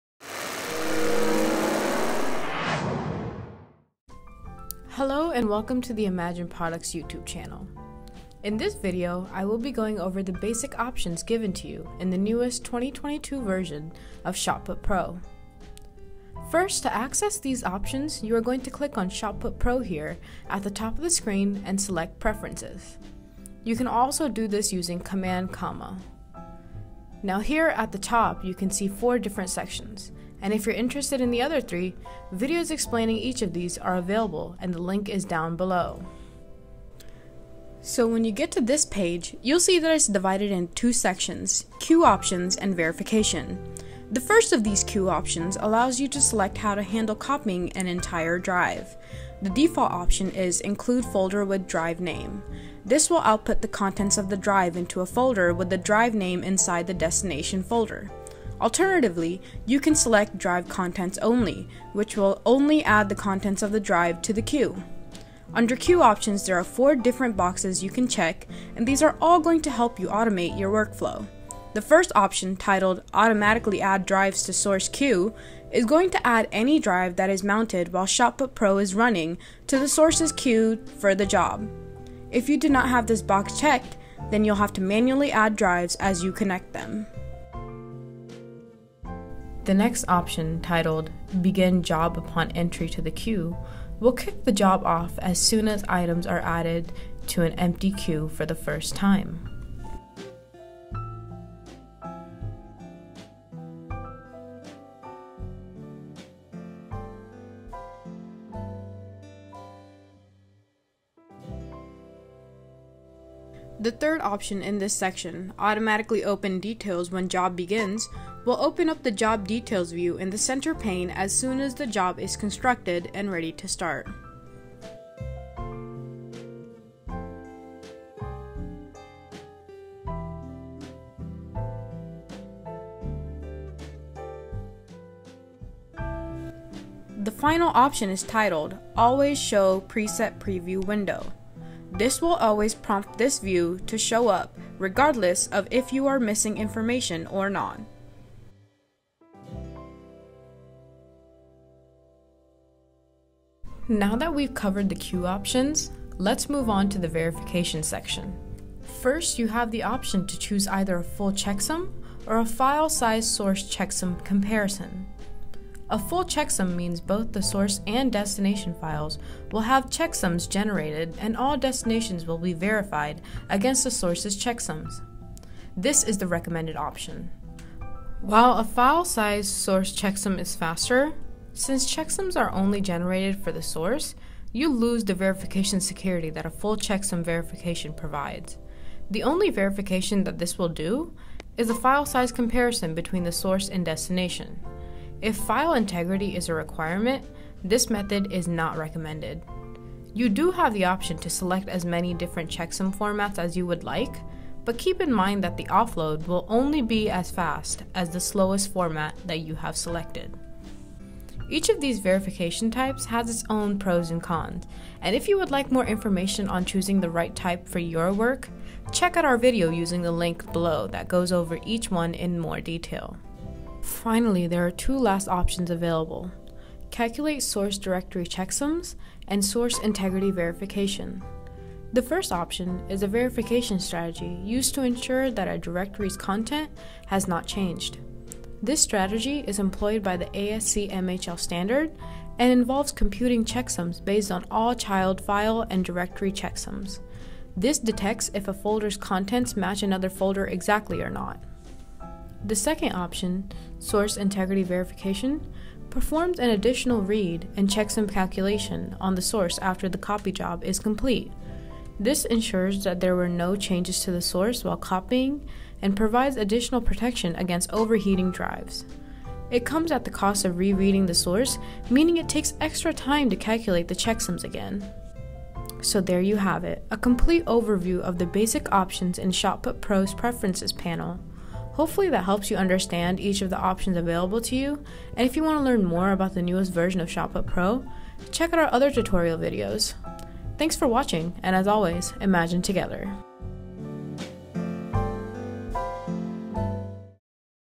Basic Options - ShotPut Pro Tutorials (macOS 2023)
In this video, we covered the Basic Options panel in ShotPut Pro®’s Preferences panel. Hopefully this video gave you the insight you needed, and if you have any more questions, check out the other videos about ShotPut Pro on our YouTube channel.